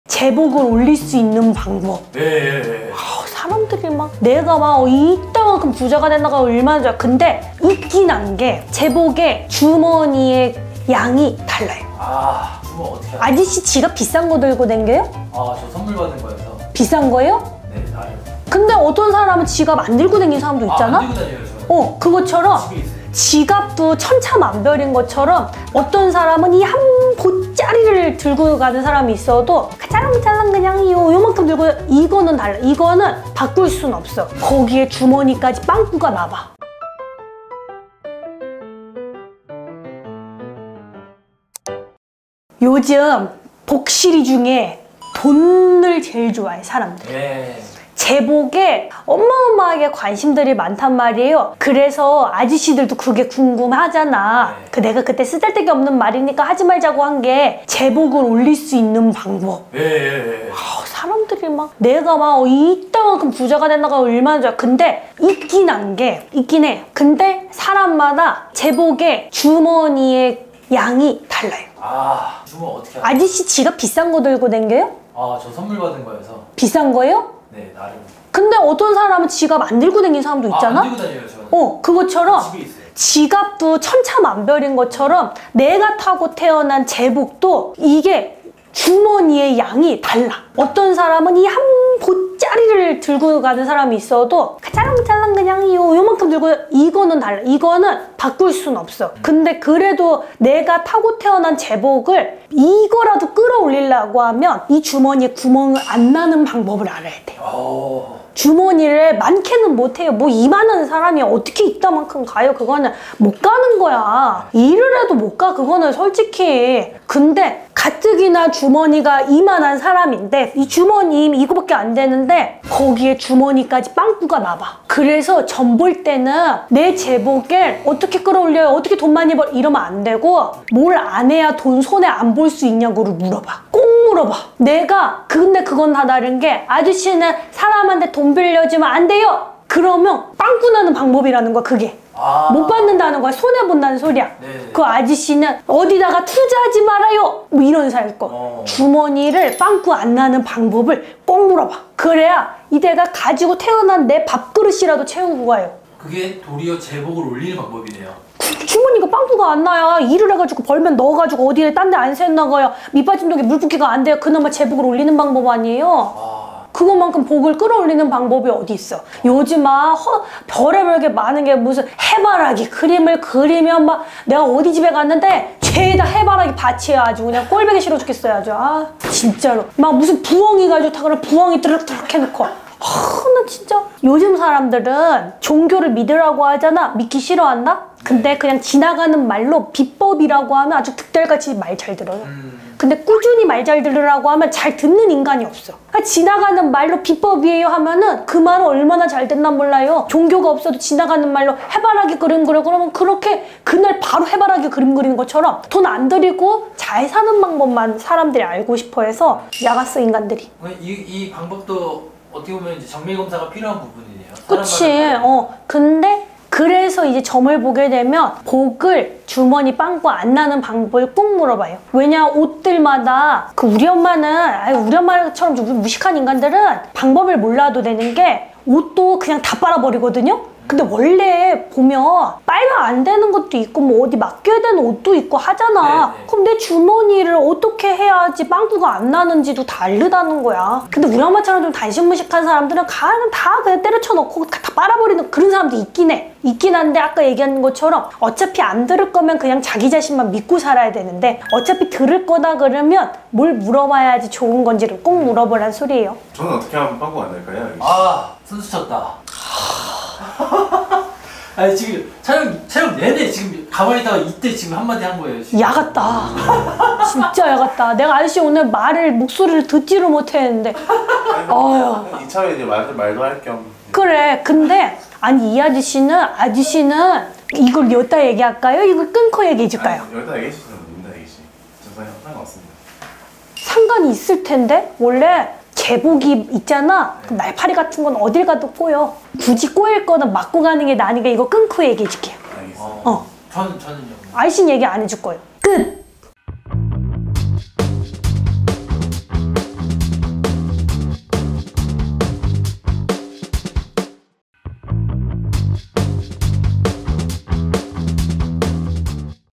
'이것'에 빵꾸가 나면 금전이 줄줄 세어나간다? 금전복,재물복 올릴 수 있는 방법 알려드릴게요!
안녕하세요. 쌍문동 애기선녀입니다.

오늘의 영상도 유익하게
시청하셨으면 좋겠습니다

쌍문동 애기선녀 선생님은 덕분TV 소속 선생님이십니다